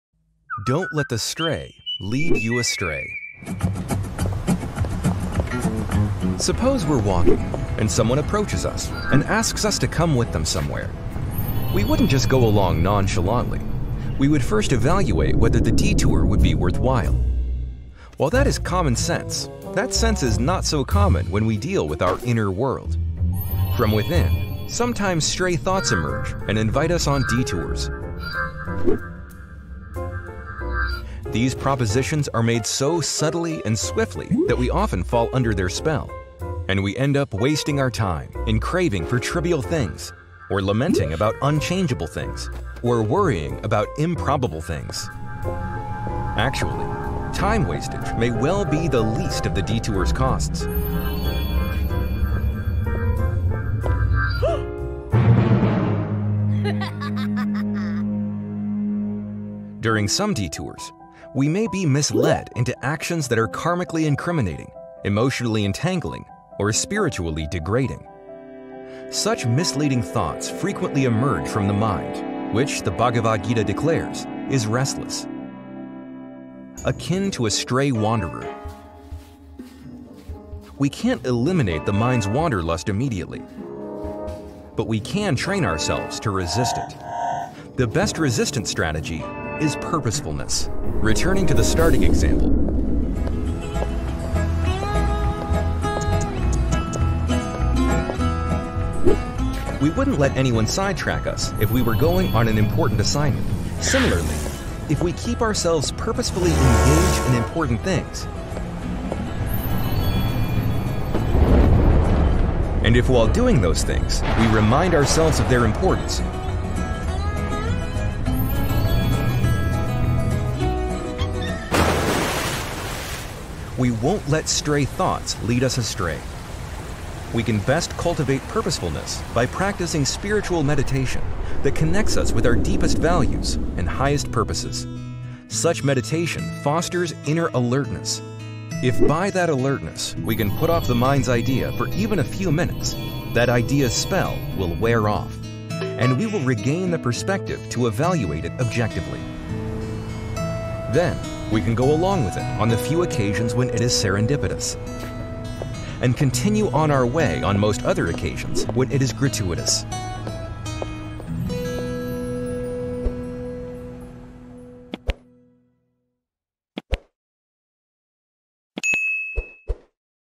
Stray Thoughts can Lead you Astray! Identifying and dealing with Stray or Random thoughts
విచ్చలవిడి ఆలోచనలు మిమ్మల్ని దారితప్పగలవు! విచ్చలవిడి లేదా రాండమ్ ఆలోచనలను గుర్తించడం మరియు వ్యవహరించడం